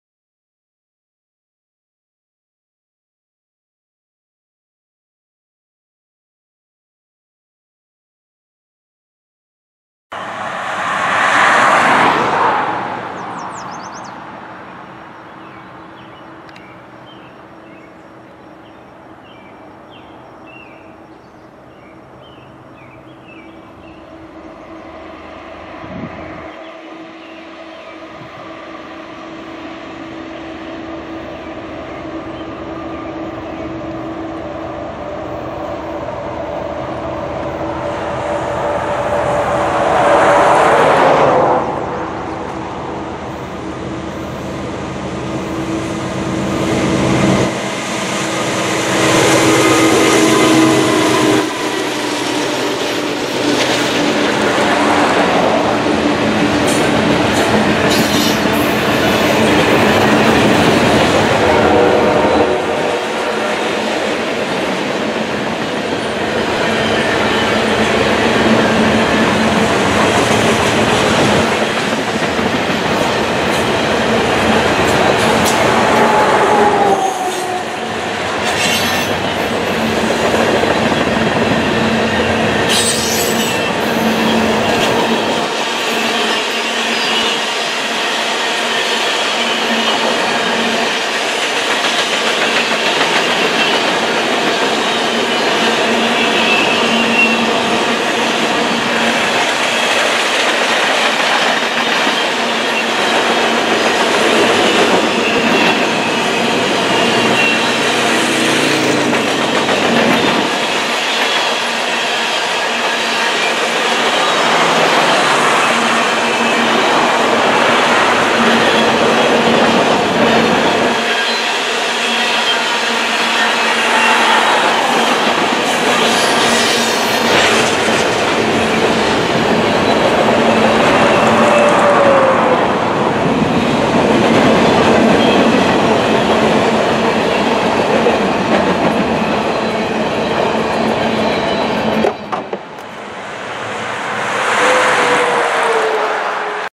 Trains BC 2016: CN 2227 East (Grain) Near Katz Canada 06JUL16 ES44DC 2227 Leading
124 down 1 to go. Finally bagging ES44DC 2227 leaves me with just 1 CN ES44DC to see (2242). Here 2227 is leading this 95 car grain train with C40-8M 2441 and SD70M-2 8853